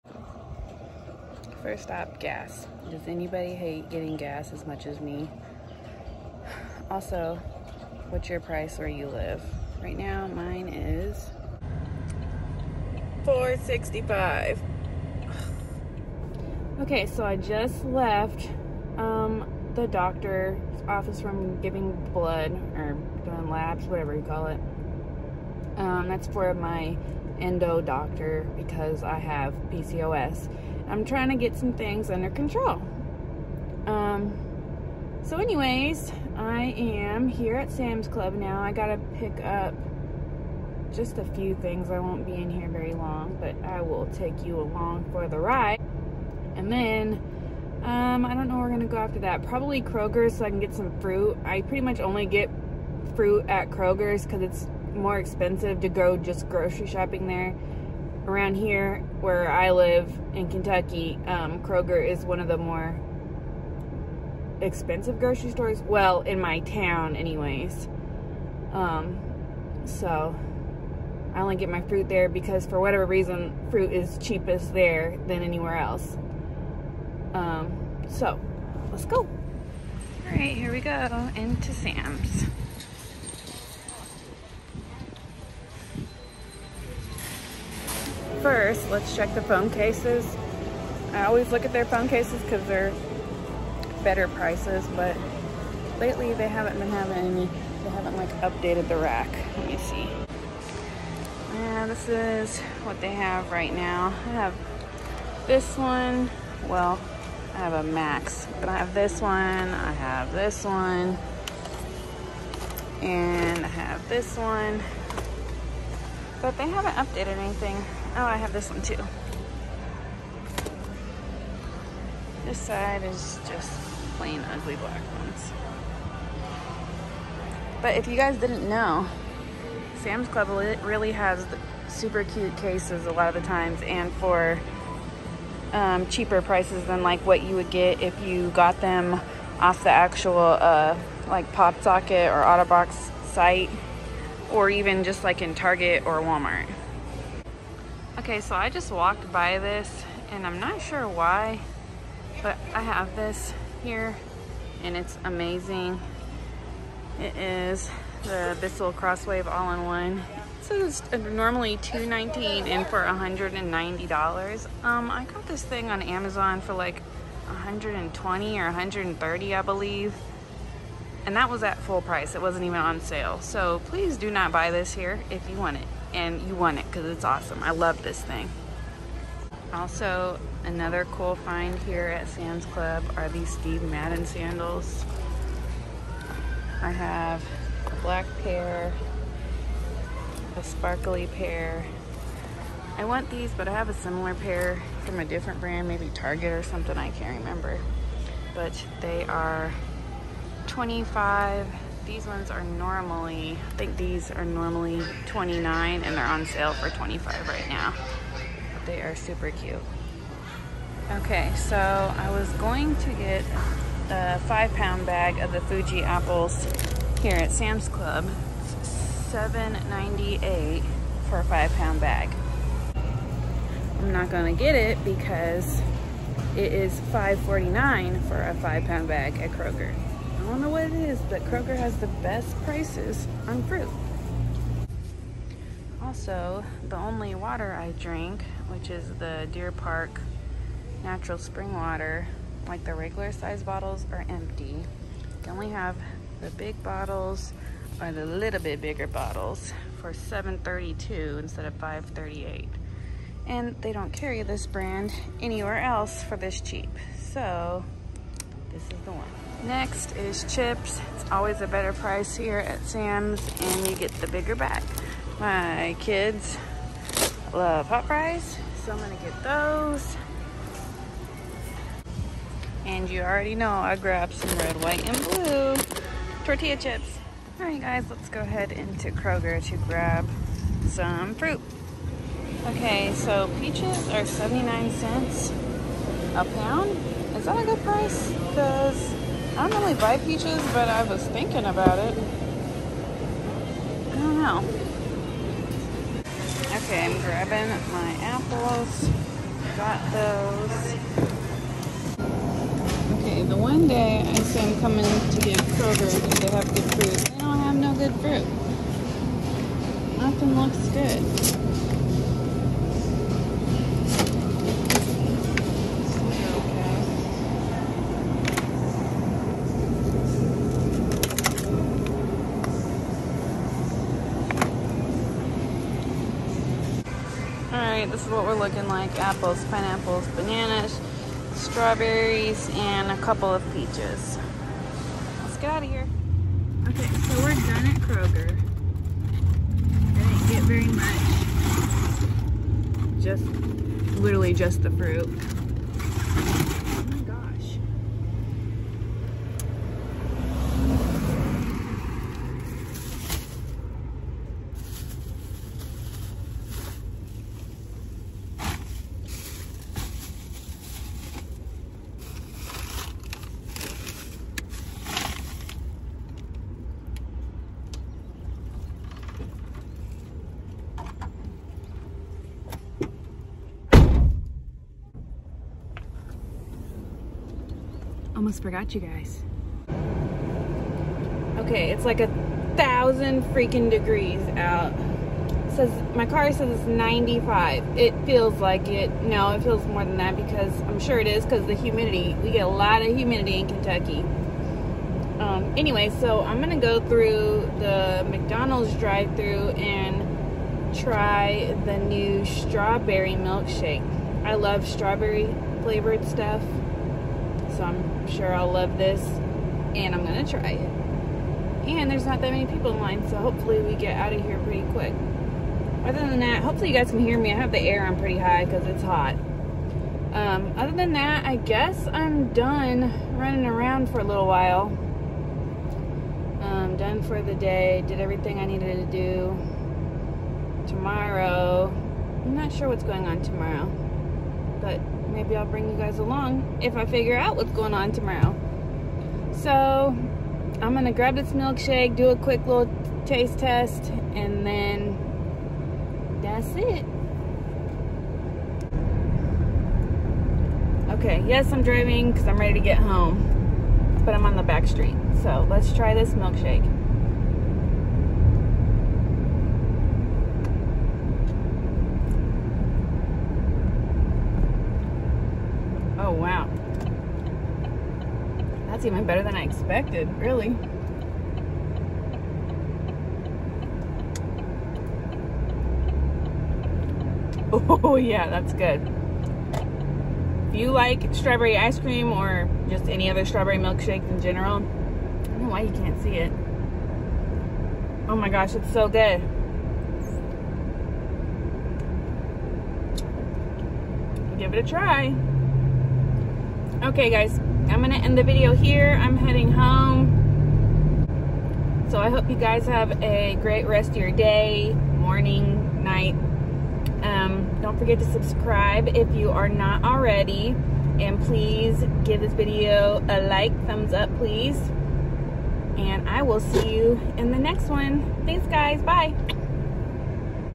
Sam’s Club + Kroger Shop With Me
Come follow me around at Sam’s Club & Kroger & a quick taste test of the new McDonald’s Strawberry Milkshake. Thanks loves